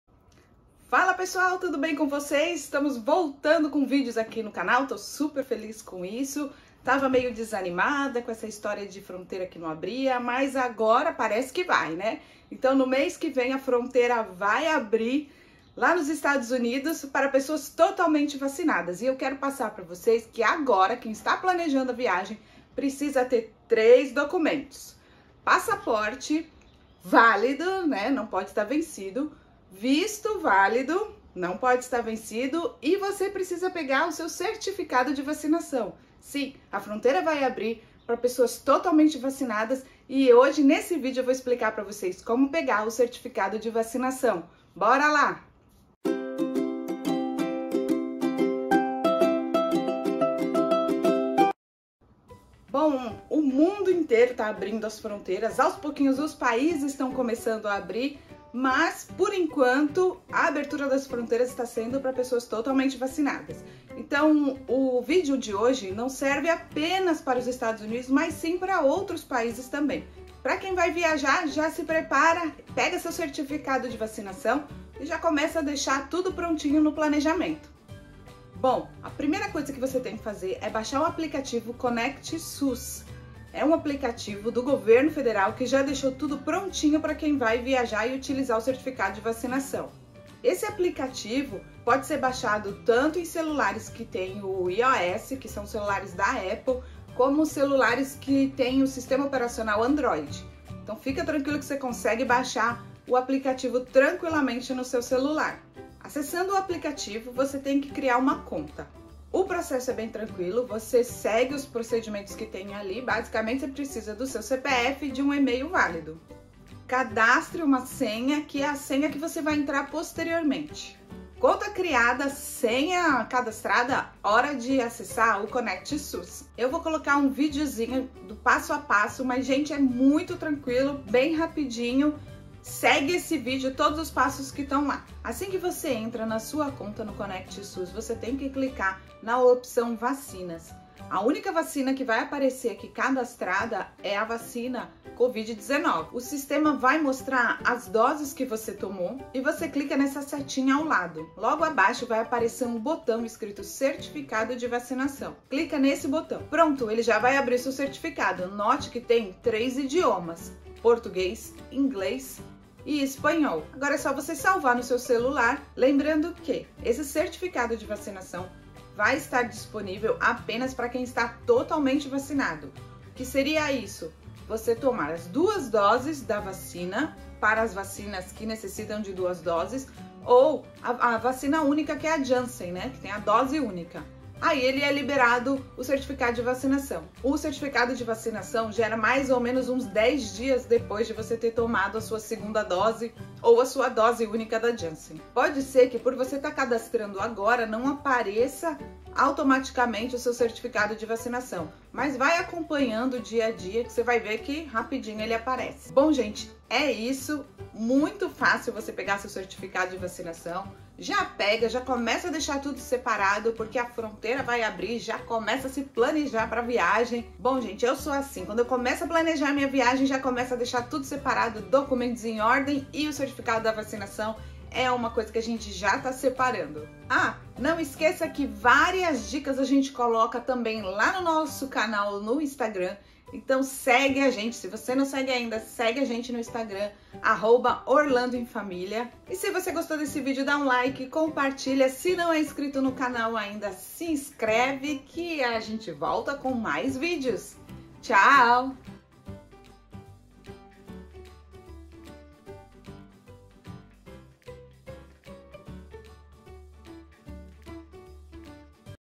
COMO PEGAR O SEU CERTIFICADO DE VACINAÇÃO PARA VIAJAR
Aos poucos as fronteiras dos países estão abrindo e no próximo mês será a fronteira dos Estados Unidos.
E, em praticamente todos os países, está sendo exigido a apresentação do certificado de vacinação . O governo brasileiro se preparou para isso e rapidamente você consegue emitir o certificado de vacinação em 3 idiomas: português, inglês e espanhol.

Mas, primeiramente, saiba que para emitir esse certificado você deve ter tomado TODAS as doses da vacina.

Aqui vou passar o passo a passo para você emitir o seu certificado e, assim, já ficar preparado para a sua viagem! Bora lá!!!